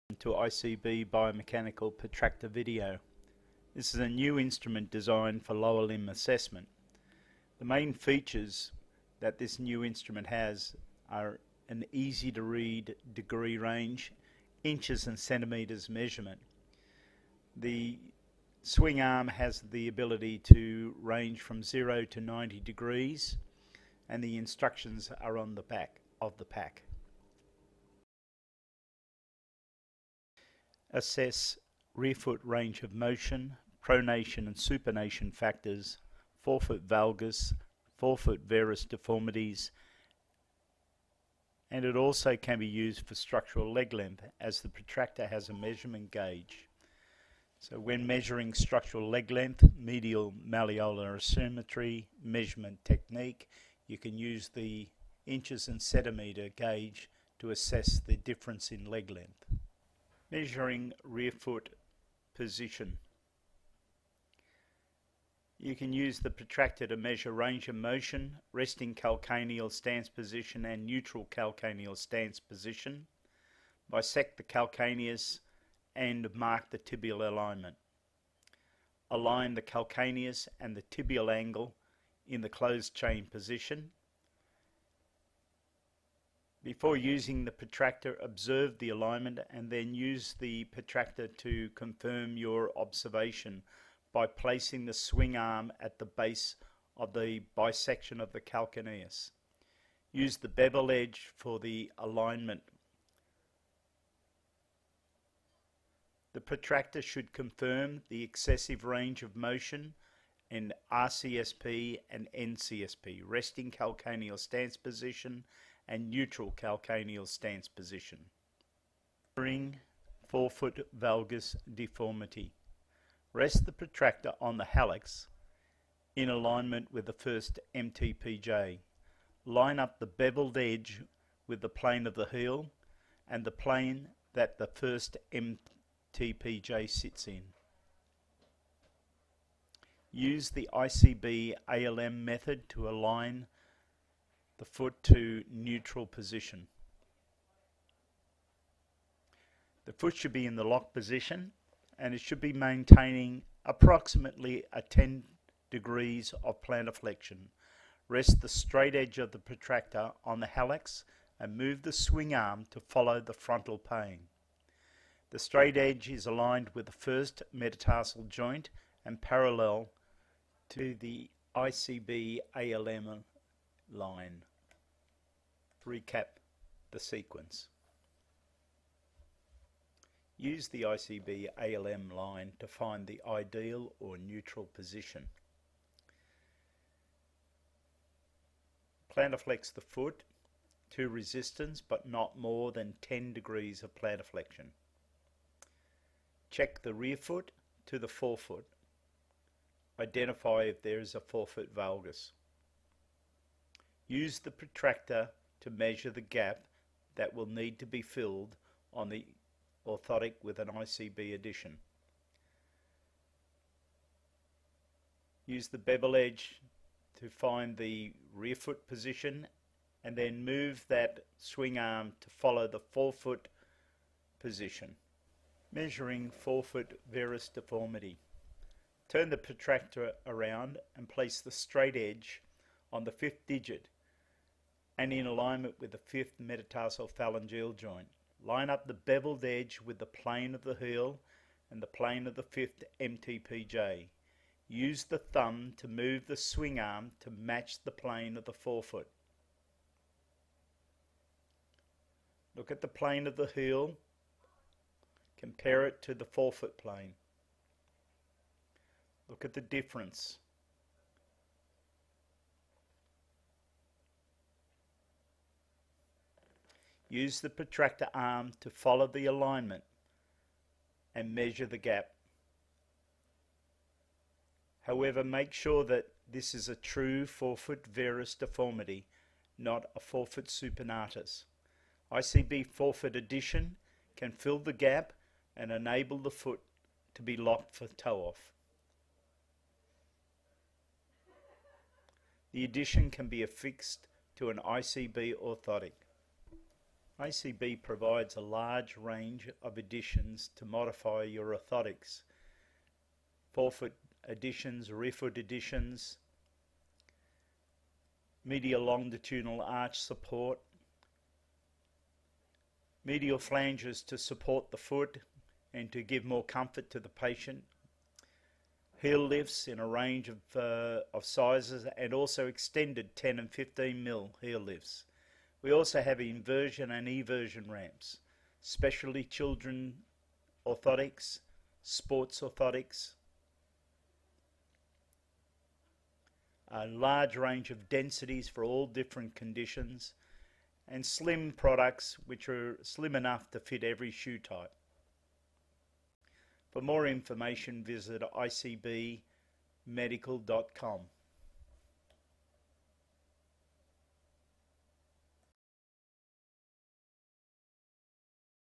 ICB Biomechanical Protractor
Assessment instrument for Lower Limb Biomechanical Assessment. Practitioner can assess rearfoot for Range of Motion, Inversion and Eversion of the calcaneus (heel) In a closed chain position (weight bearing). The instrument can also be used in assessing Forefoot Valgus, Forefoot Varus and Structural leg length as it has a inches and centre meter measure when assessing in supine. or find your nearest Distributor. made from sturdy plastic features clear 0 to 90 degree measurement range.